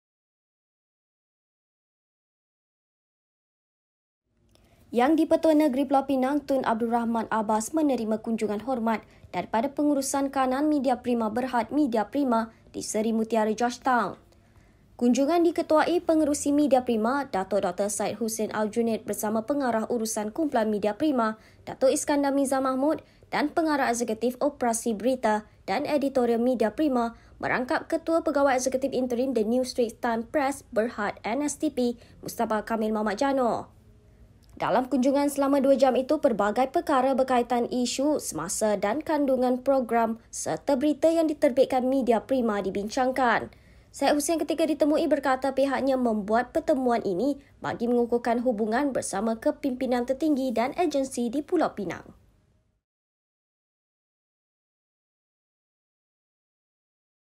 Media Prima buat kunjungan hormat ke Pulau Pinang
Yang Dipertua Negeri Pulau Pinang, Tun Abdul Rahman Abbas menerima kunjungan hormat daripada pengurusan kanan Media Prima Bhd (Media Prima) di Seri Mutiara, Georgetown.

Kunjungan diketuai Pengerusi Media Prima, Datuk Dr Syed Hussian AlJunid bersama Pengarah Urusan Kumpulan Media Prima, Datuk Iskandar Mizal Mahmood dan Pengarah Eksekutif, Operasi Berita dan Editorial Media Prima, merangkap Ketua Pegawai Eksekutif Interim The New Straits Times Press (M) Bhd (NSTP), Mustapha Kamil Mohd Janor